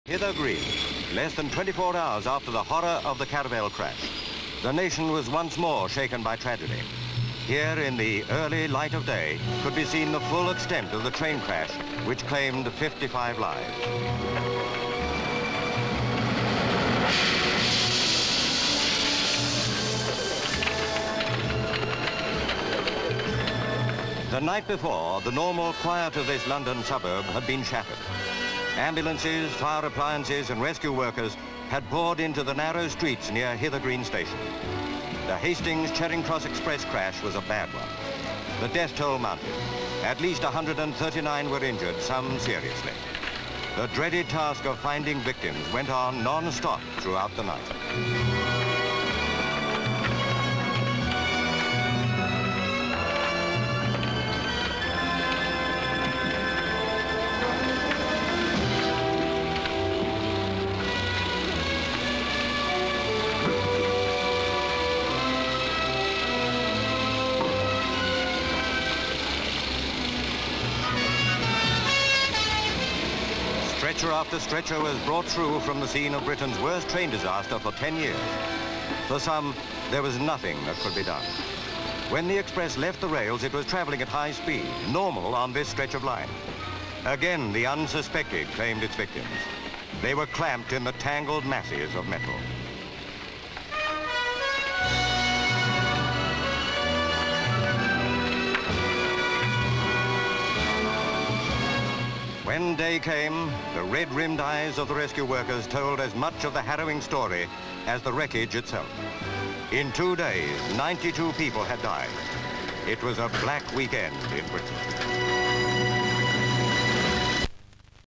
Train Crash Aka 55 Die In Train Derailment (1967)
No title.  Train crash - 55 die in train derailment at Hither Green, London.

L/S shot train crash site.  M/S overturned carriages.  L/S wreckage.  M/S ditto.  M/S seats and debris in centre of track.  Pan up to wrecked carriages.  C/U man cutting away with acetylene lamp.  L/S wrecked dining car, showing one side completely ripped away.  M/S ditto.

Next section filmed at night.  Various shots ambulances waiting in street at Hither Green.    C/U sign: "Hither Green Continental Freight Depot".   Various shots of rescue workers getting injured and dead out of wreckage.  M/S Salvation Army women serving tea to rescuers.  Various shots of rescue workers bringing out dead and injured.  Various shots ambulance men carrying dead on stretchers to ambulance.  M/S ambulance driving away. Various shots rescue workers taking dead away on stretchers.  L/S wrecked coach.  M/S ditto.  L/S ditto.  Interior shots of wrecked coach.  Various shots coaches on their sides.

Various daytime shots of crash site.
 FILM ID:2045.28